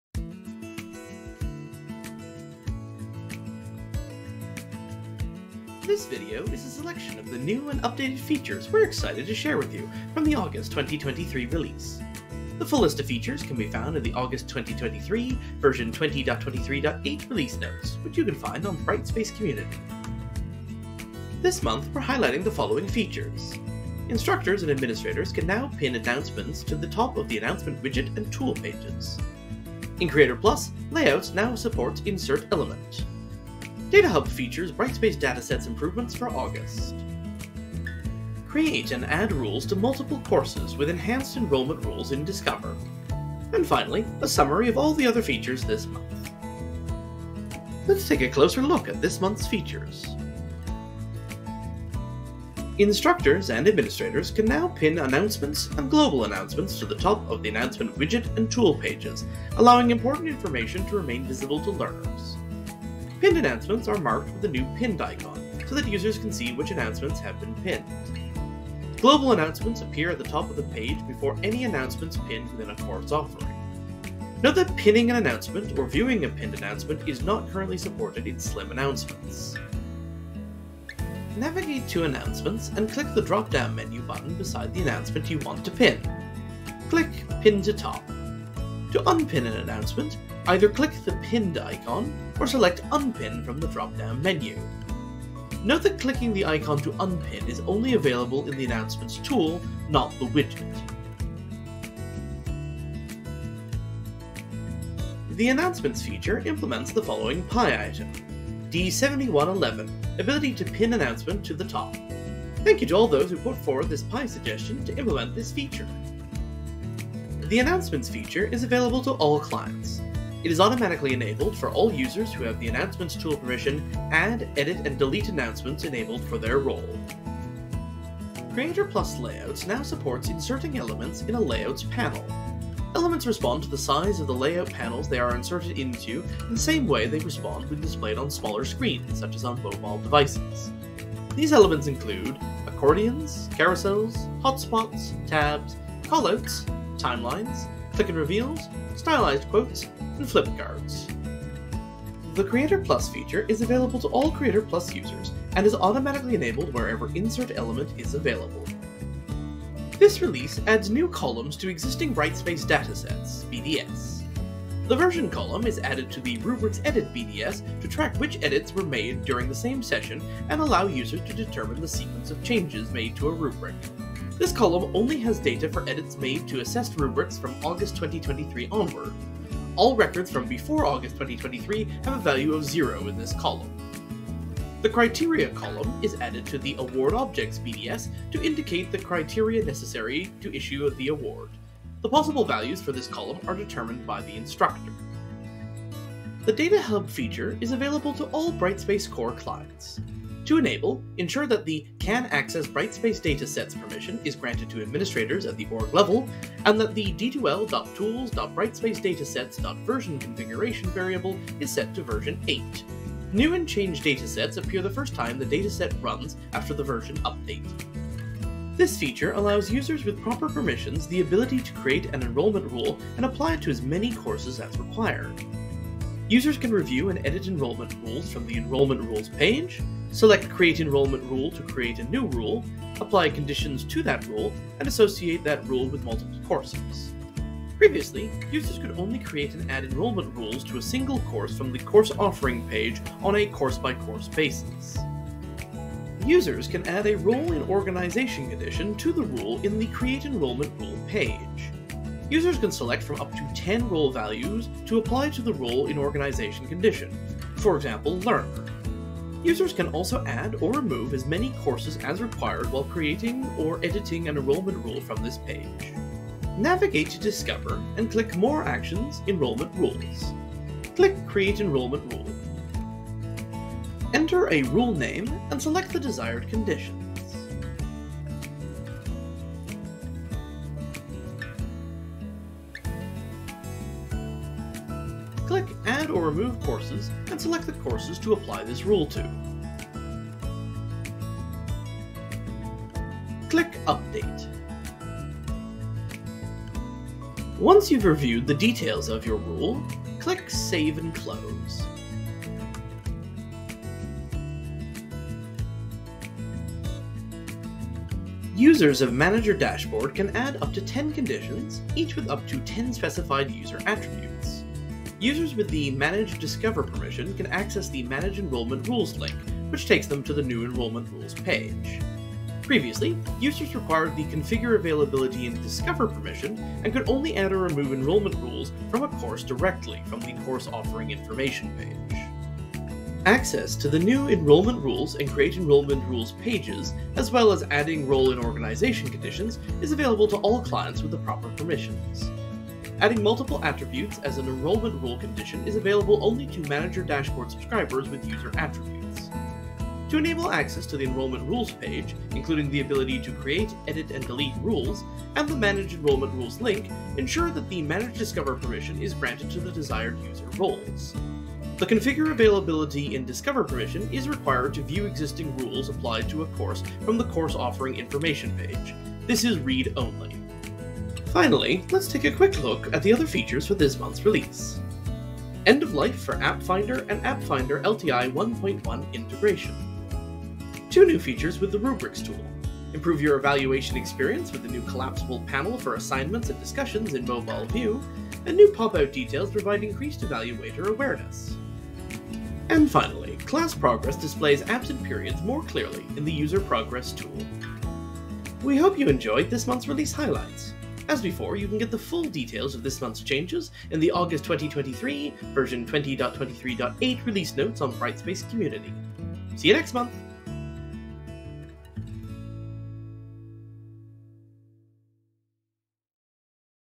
August 2023/20.23.8 Release Highlights Video
This video highlights some of the new and updated features of Brightspace 20.23.8. The full list of features can be found in the August 2023 / 20.23.8 Release Notes on Brightspace Community.

-- Release Highlights Contents --
00:00 - Introduction
00:20 - Features List
00:52 - Announcements (Pin announcements to top of Announcement widget and tool)
02:07 - Creator+ (Layouts now supports Insert Element)
02:39 - Data Hub (Brightspace Data Sets improvements for August)
03:38 - Discover (Create and add rules to multiple courses with enhanced Enrollment Rules)
06:10 - Other Features Summary
06:38 - Outro